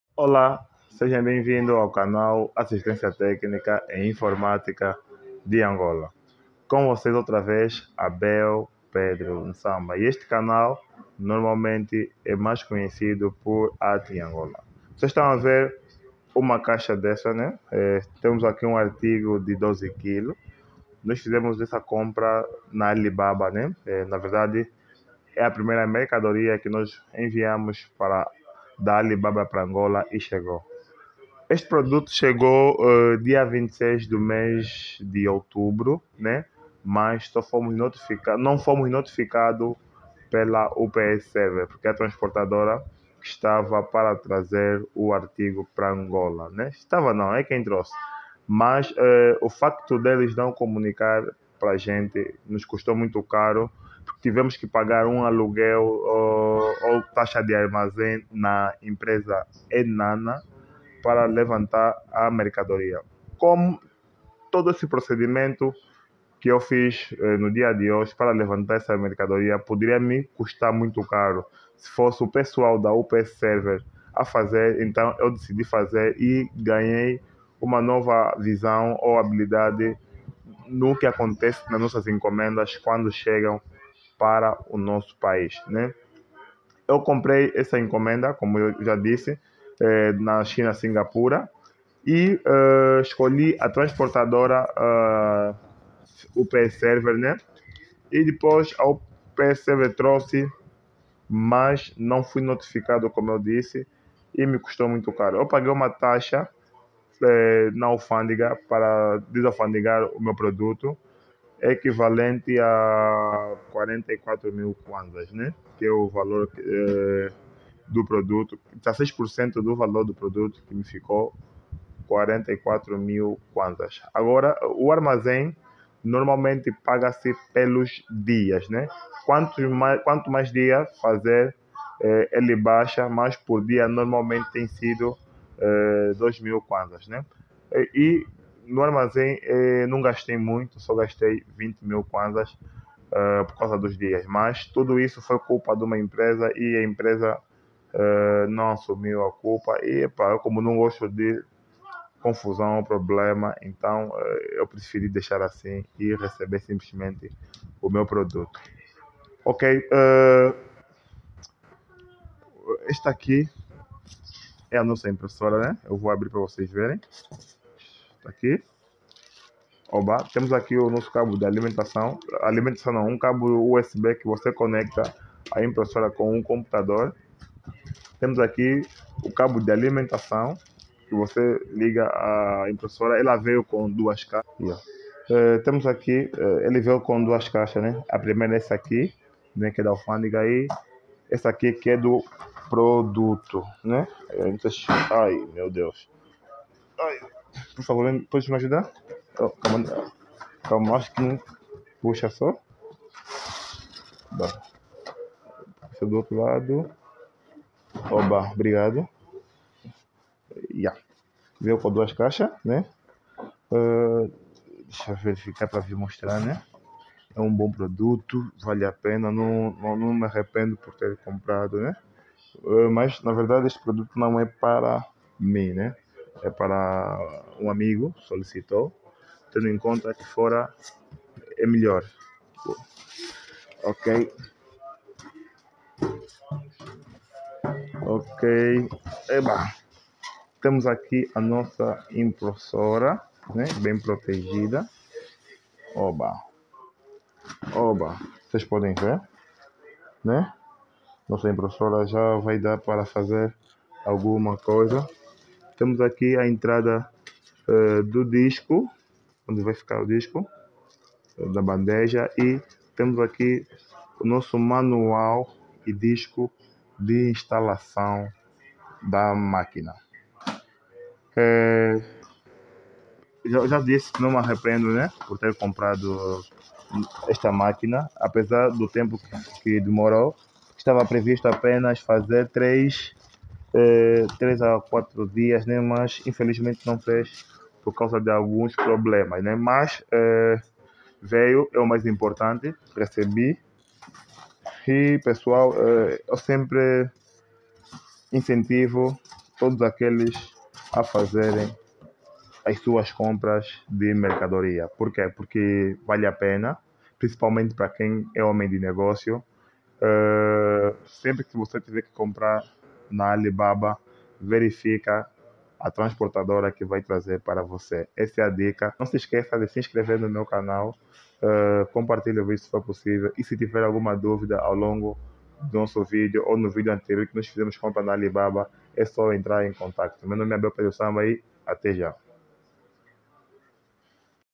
Chegou a nossa Primeira Compra da ALIBABA da China Para Angola, saiba tudo assistindo.
Faça parte da nossa comunidade  no telegram :

Compra na Alibaba

Grupo destinado para troca de experiências no que concerne as tecnologias de informação.